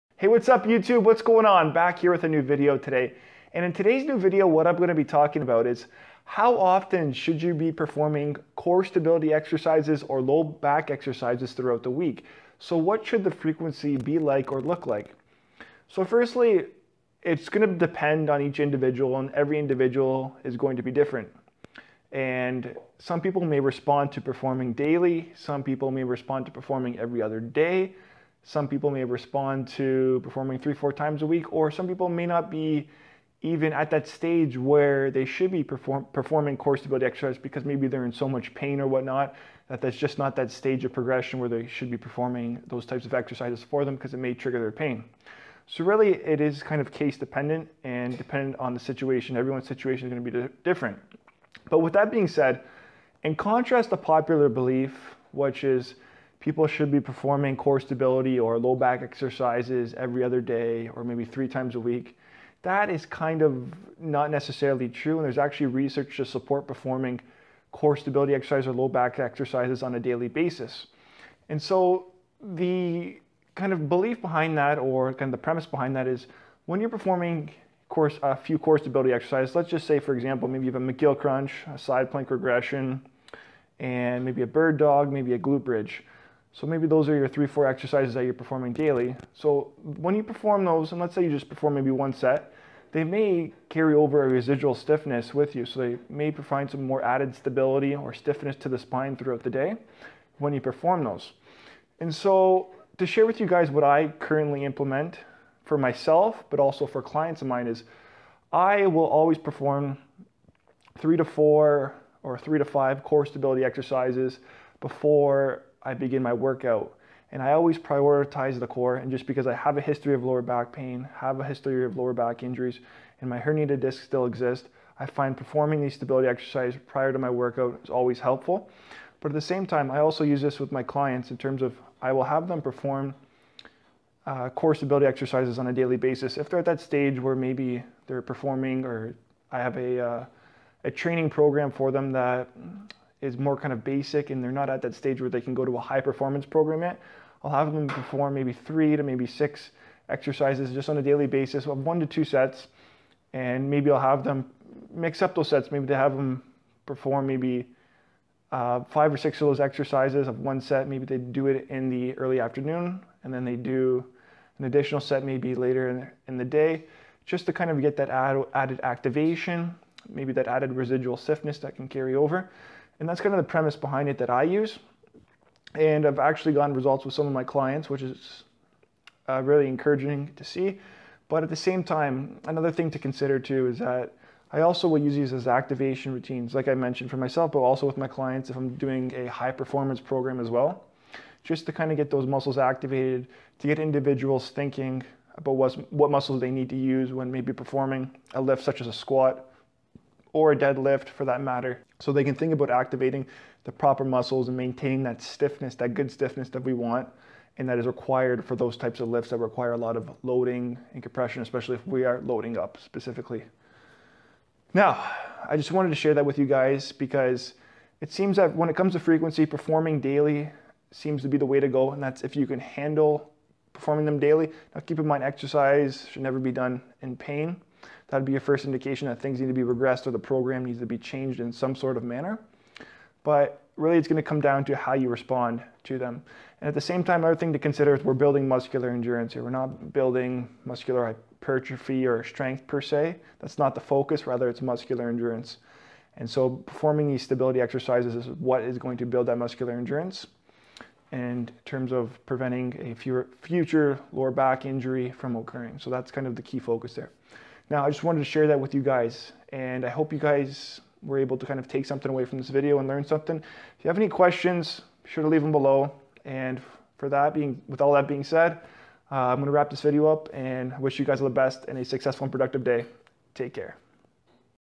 How Often Should You Perform Core Stability and Low Back Exercises?
Contrast to popular belief, many people believe that they should perform core stability or low back exercises every other day. However, research from Mayer et al., (1985) showed that performing daily low back exercises were beneficial in helping individual's following an industrial low back injury return to work compared to a group of patients that were denied treatment from their insures. Based off this research, we see that there is support for daily core stability/low back exercises in people with lower back pain. Now, that doesn't mean everyone should go perform daily core stability/low back exercises, since everyone may respond different. Some people may respond to daily, some every other day or some may not be at the point to perform core stability/low back exercises since they are in so much pain and it would be best to wait until the pain comes down. However, the theory behind performing these daily core stability/low back exercises is that they will create a 'residual' stiffness in the spine that may last for hours throughout the day. This residual stiffness may help spare the spine when performing certain movement or tasks that specifically may involve loading the spine. Additionally, when performing daily core stability/low back exercises, it's important to understand that we are developing muscular endurance and not muscular strength. Two distinctly different concepts that require different training programs. Muscular endurance would likely respond better to daily since we are always using our core stabilizer muscles during daily tasks such as walking, running, standing, etc. On the flip side, we can't train muscular strength everyday since we would only overload the body, accumulate fatigue and in a matter of time create an injury.

Further, based on my personal experience and my history of lower back issues, I routinely perform 3-4 core stability exercises prior to each and every workout I perform. I've personally found this to be especially helpful with my workouts as I feel I can get a better understanding of which core muscles I need to activate when performing more advanced lifts such as squats or deadlifts.

Lastly, when it comes to my personal clients with lower back issues, daily core stabilization exercises will almost always be implemented if they don't cause pain. Additionally, for many of my high performance clients they will perform core stabilization and activation exercises prior to their workout. This is to ensure they are prepared and know what muscles they need to stiffen when it comes exercises such as the deadlift or squat.

References:

Mayer TG, Gatchel RJ, Kishino N, et al. 1985 Volvo Award in Clinical Sciences. Objective assessment of spine function following industrial injury: a prospective study with comparison group and one-year follow-up. Spine 1985;10:482–93.

Your comments inspire me to keep this channel going, so please take a second and say ‘What's up’ ❤ .

Questions!?